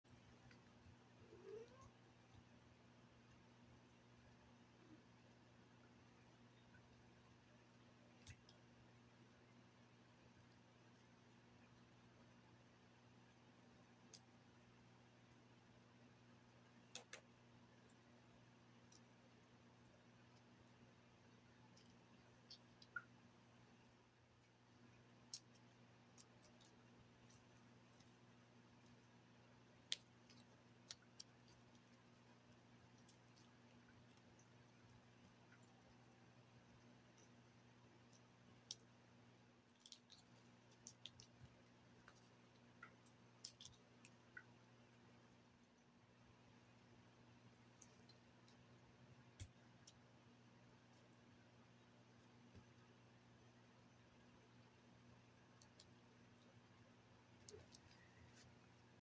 Kitten Plays with Its Favorite Toy
Peaches enjoys playing with the feather teaser toy.